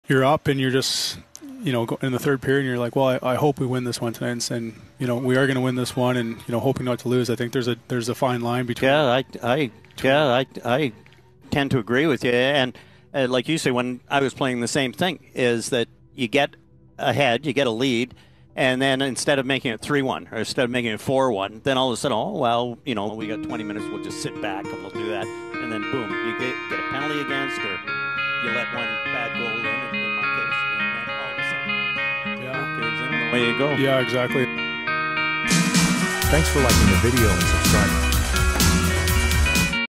John Garrett CUTS OFF Canucks Luke Schenn Question
I love Cheech but he needs to be called out on this. Just making fun of the awkward moment.
Official GLOVEY T-Shirt NOW AVAILABLE!